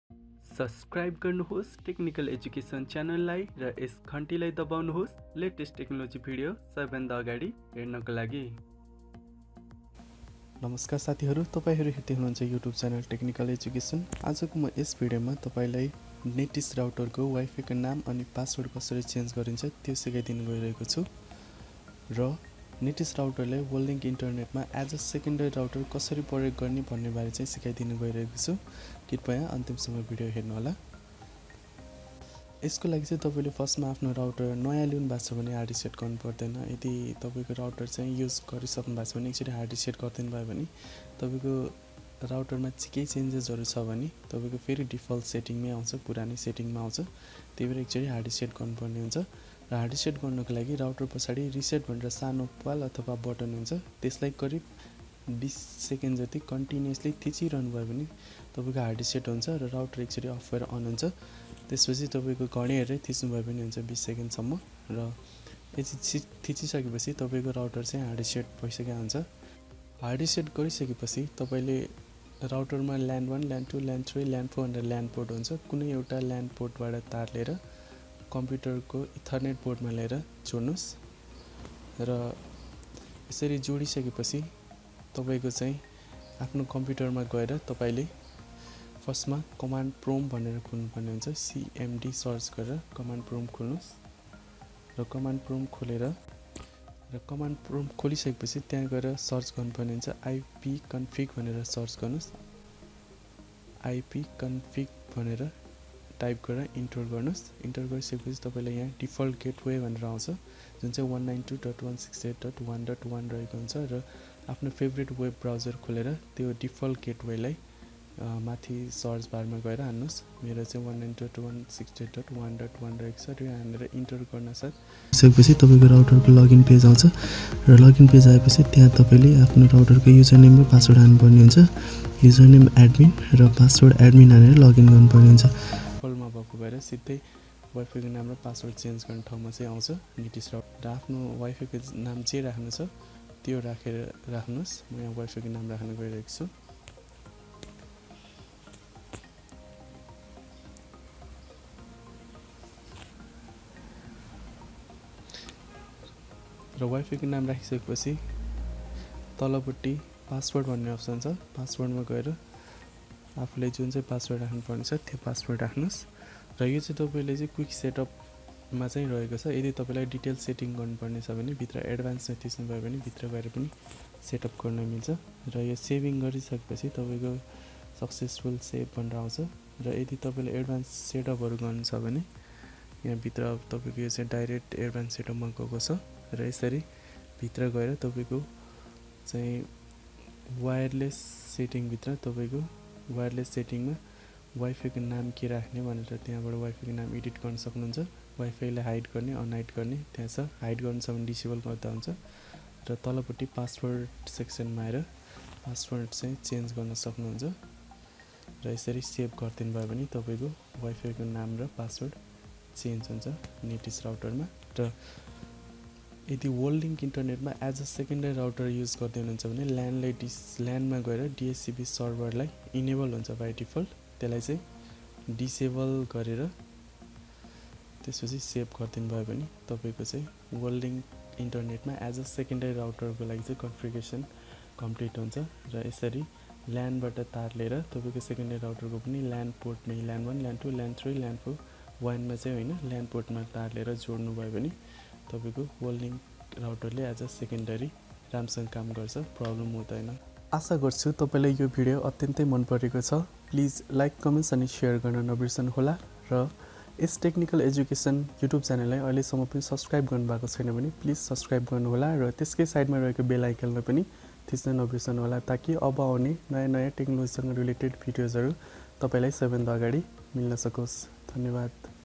How to Configure Netis Router configuration for Worldlink Internet 2020?Worldlink wifi काे setting.
This video provides information about how to setup netis router wifi name and password and setup for worldlink internet connection.